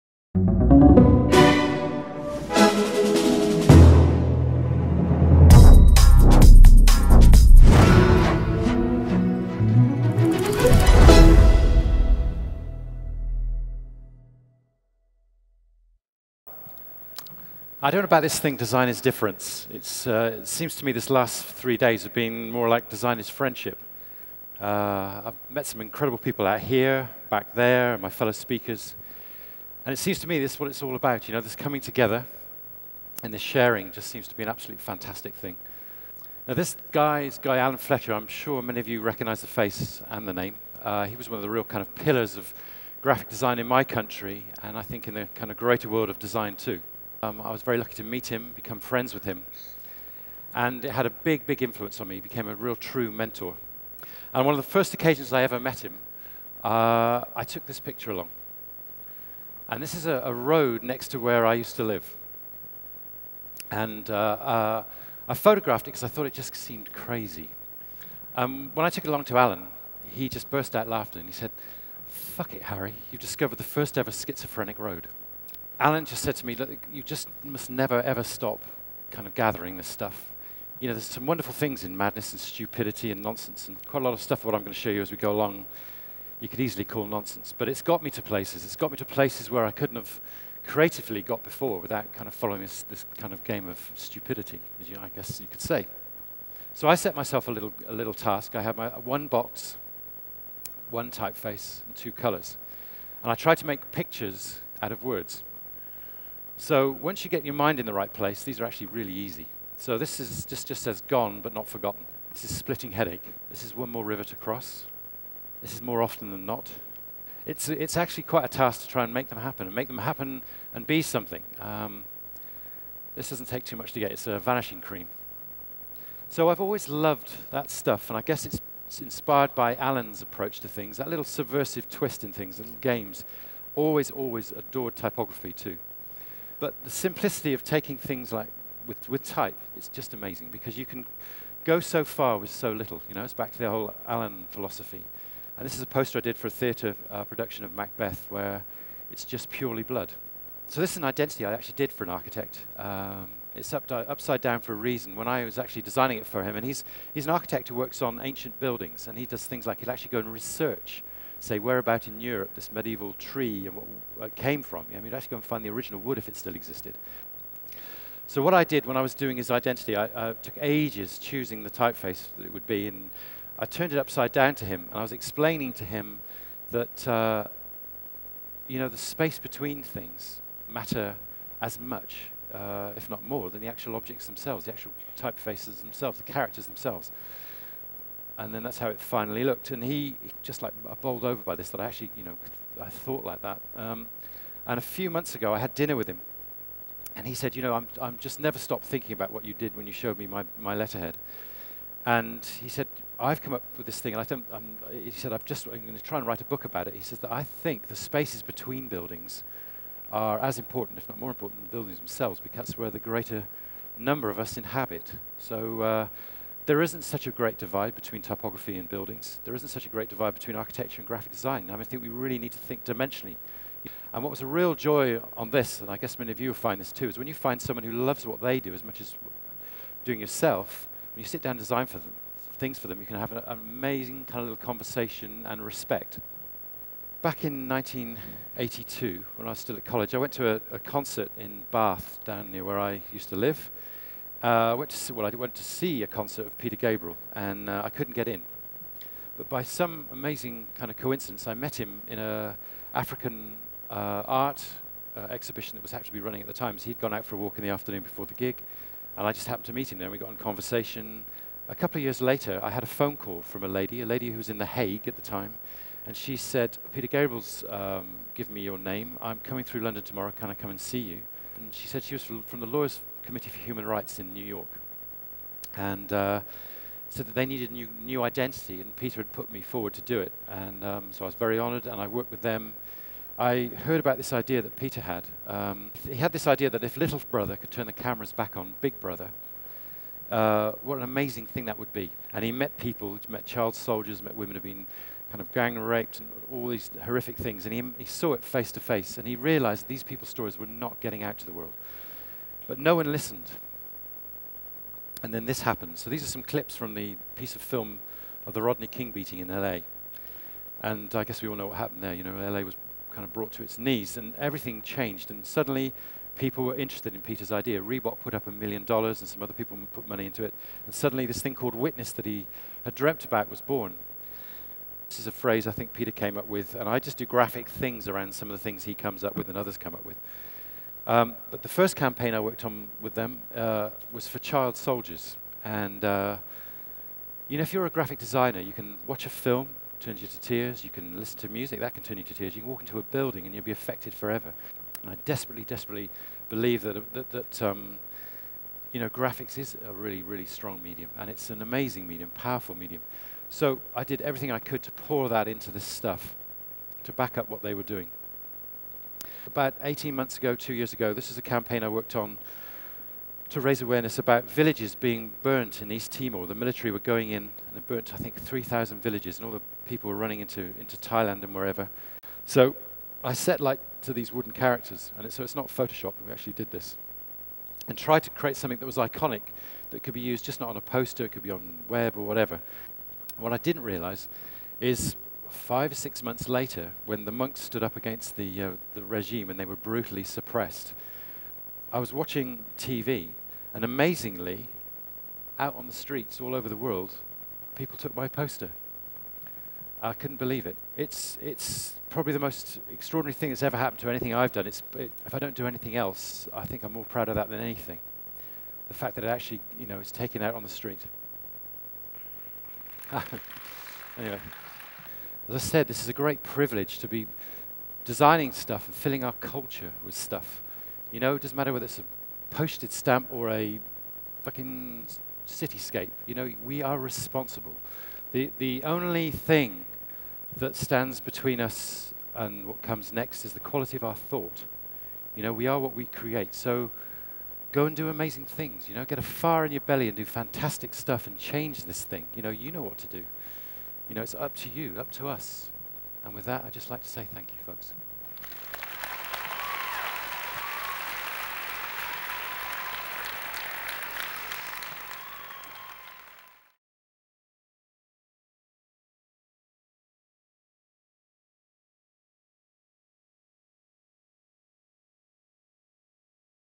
Harry Pearce reflects on the "silly" things - agIdeas 2009
The now current partner at Pentagram Design graced the stage at the international design forum presented by agIdeas in 2009. It was here that he shared the process behind his his award winning work and the the important influences that lead him to his success. Gain insight into a new way of perceiving the creative process and the outcomes with this leader of design.

He excels in the creative process that leads to beautiful graphic design, and he shares how he owes it to more then just pen and paper.